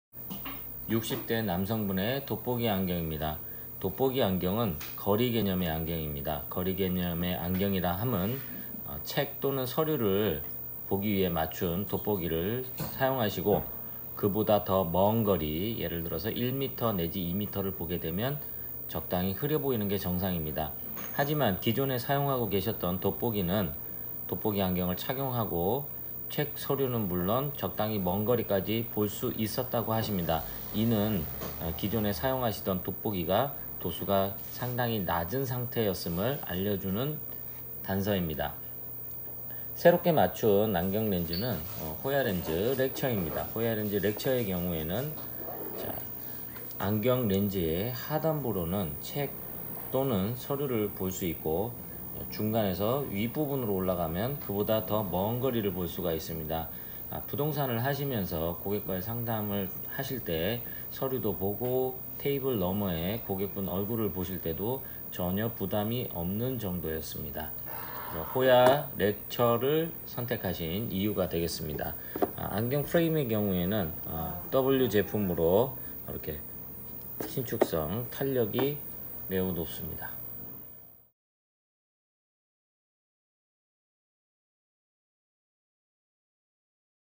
공인중계사 기능성 돋보기 호야렌즈 렉처
60대 공인중계사입니다.
기존 돋보기 안경은 근거리를 볼 때 불편하여 새롭게 돋보기 처방을 받았습니다. 근거리는 잘 보이지만 기존 돋보기로 보이던 3미터 전후의 거리는 흐려 보입니다.

기능성 돋보기의 경우는 책, 서류는 물론 컴퓨터 모니터 그리고 사무실에서 고객과 상담하며 테이블 너머의 고객과 아이컨택을 하기에 전혀 무리가 없습니다.

또한 편안한 자세로 컴퓨터 모니터를 볼 수 있기에 평상시 안경착용을 꺼려하시고 근거리의 넓은 시야를 원하시는 경우에 추천됩니다.

마이더스 안경원에서 누진다초점 렌즈는 물론 다양한 타입의 기능성 렌즈를 착용해보신 후 결정하셨습니다.

근거리에서 넓은 시야를 원하시는 경우 기능성 렌즈
(현재 동영상 내용의 포스팅입니다.)

근거리 악보 볼 때만 사용하시지만 원거리 흐림도 중요하여 누진다초점 렌즈를 선택하신 케이스입니다.

정해진 답은 없습니다.
사용자의 라이프스타일과 니즈에 맞춰 불편함을 해결해 가는 과정이 바로 '비전 컨설팅'입니다.